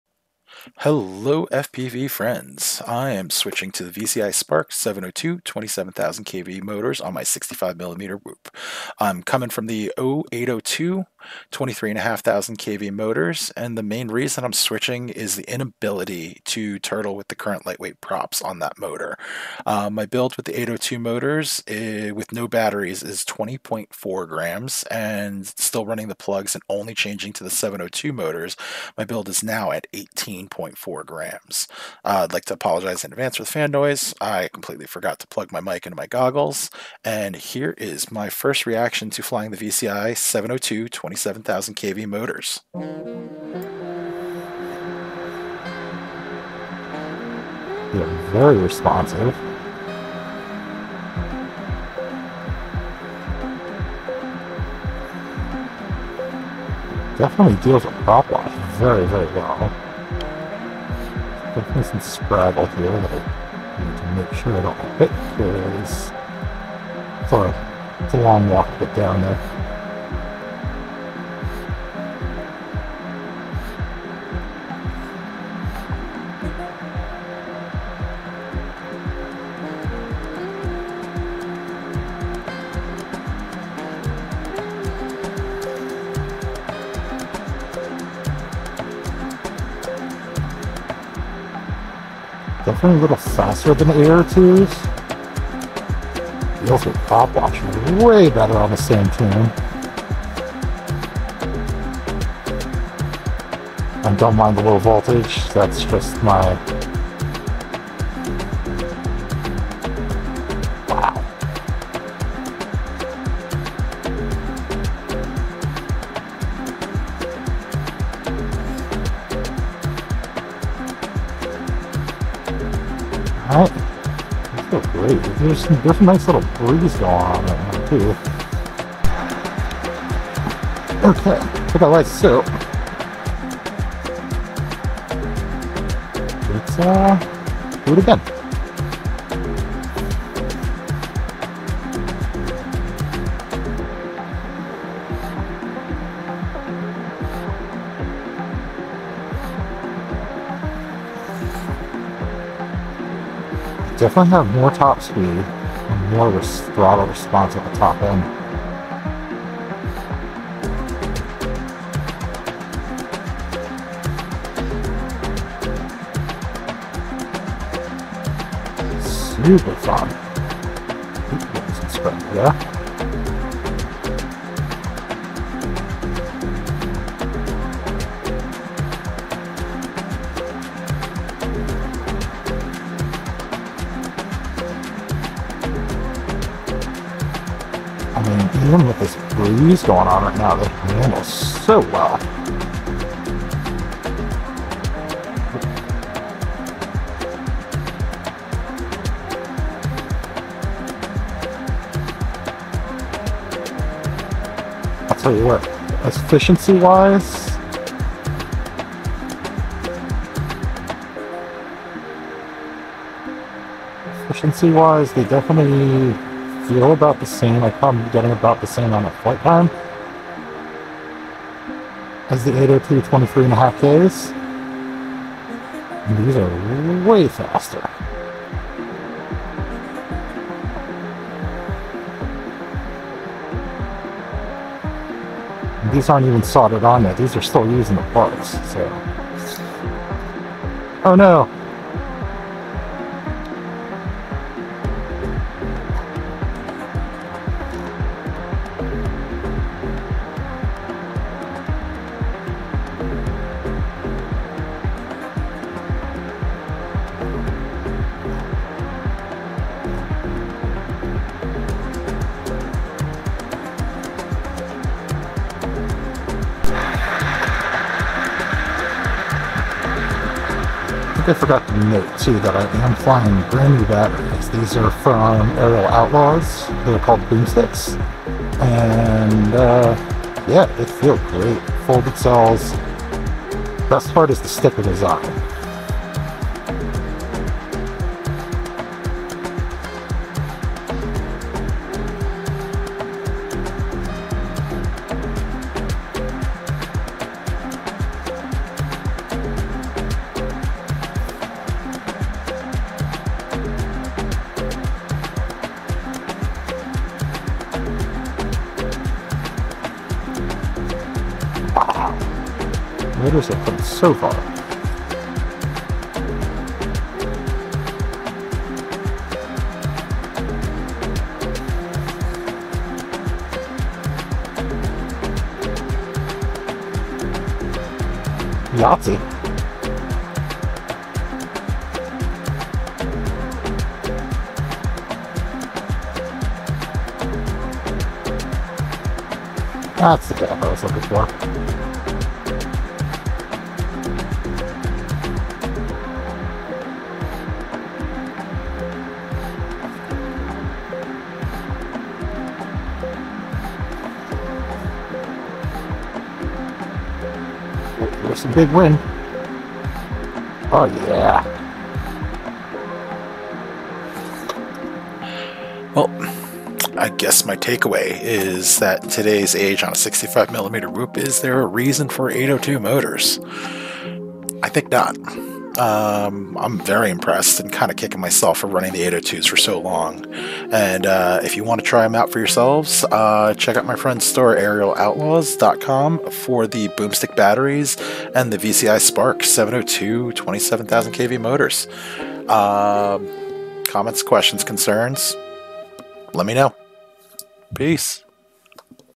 VCI Spark 702 27K motors first 3 flights/ initial reaction
First 3 batteries flow with the VCI Spark 702 27K motors on a 65mm whoop.

Build:
BetaFPV Meteor 65 Frame (Unmodified)
HappyModel AIO FC/ESC/Receiver/VTX
VCI Spark 702 27K Motors
Gemfan 1219S Props
Pinch Camera with IR Filter 45°
Boom Stick 1S 300mah BT2.0 battery.